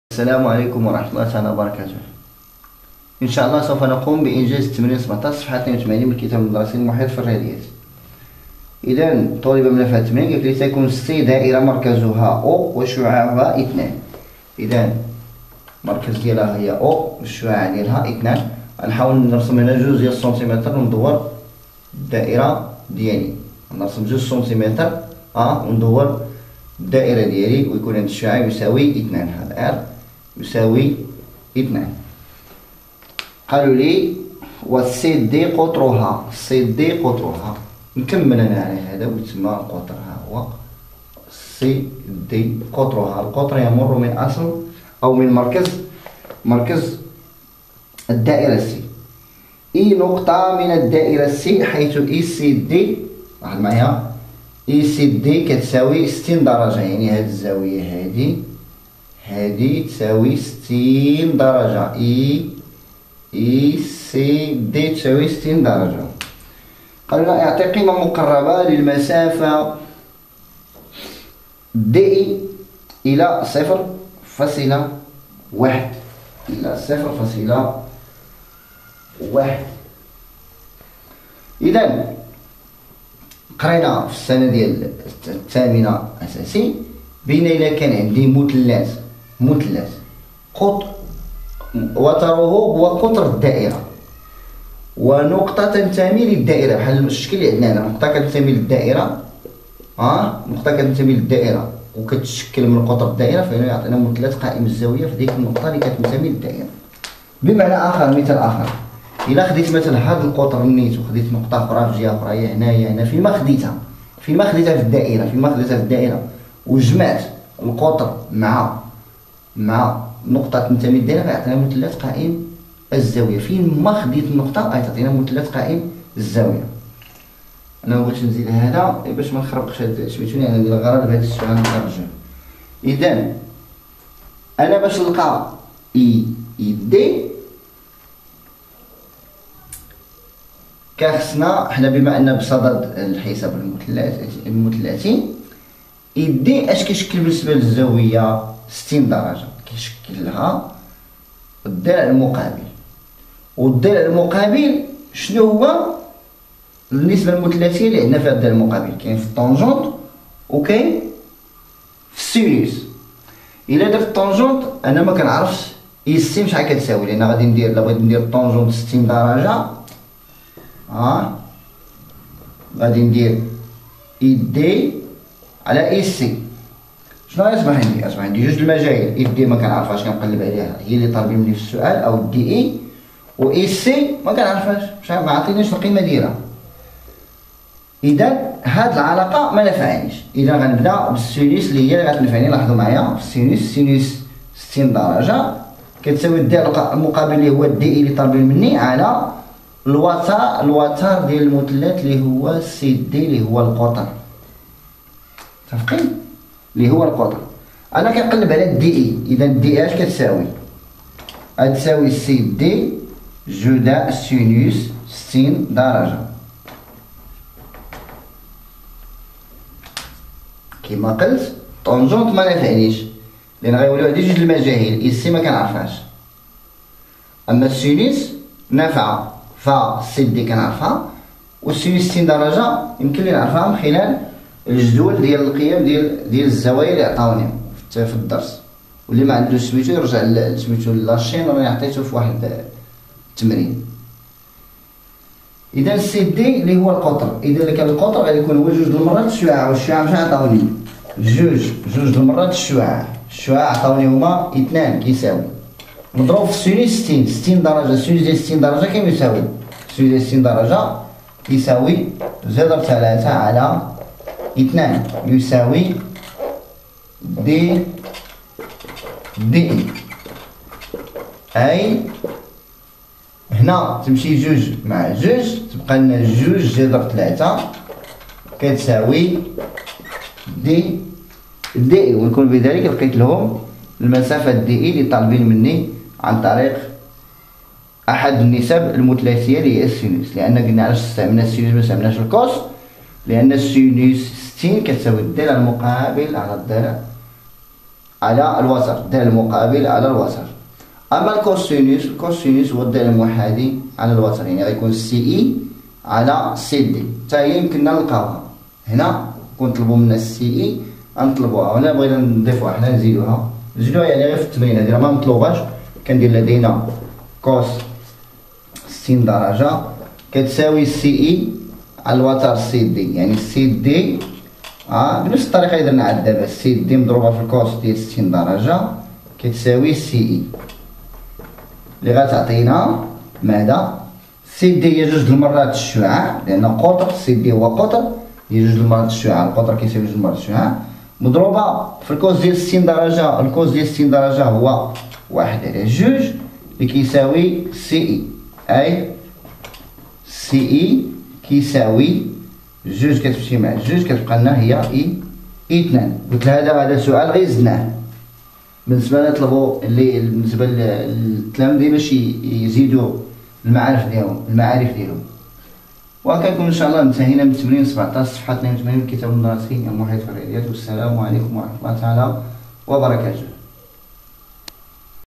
حل التمرين 17 الصفحة 82 من الكتاب المدرسي المحيط في الرياضيات الثالثة اعدادي الحساب المثلثي
الأستاذ دحميش رشيد Dahmich rachid